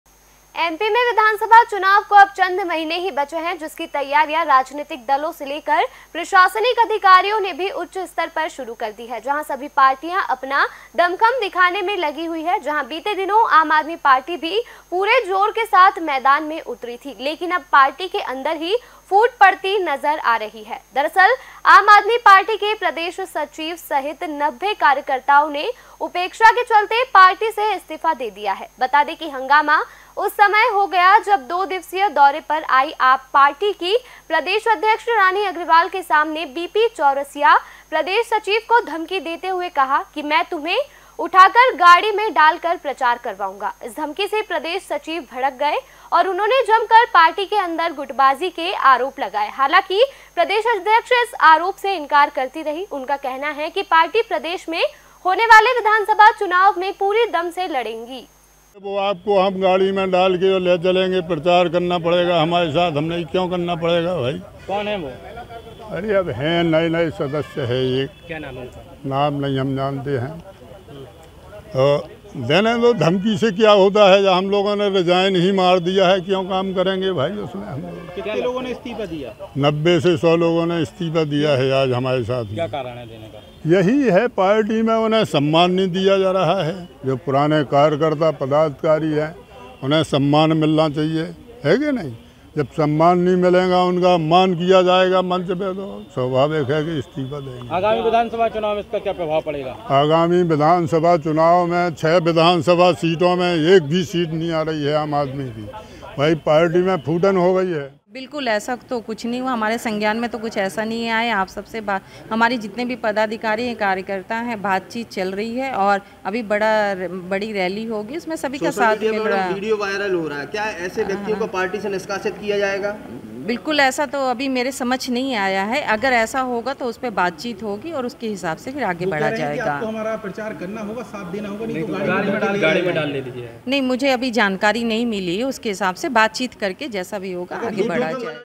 AAP में बड़ी गुटबाजी, कार्यकर्ता एक दूसरे को देने लगे धमकी! | ACN Bharat
विधानसभा चुनाव 2023 की तैयारी से पहले ही आप कार्यकर्ता आपस में भीड़ गए। दरअसल, छतरपुर जिले के दौरे पर आई आप पार्टी के प्रांतीय अध्यक्ष रानी अग्रवाल, आप पार्टी के कार्यकर्ता सर्किट हाउस में आपस मे भीड़ गए।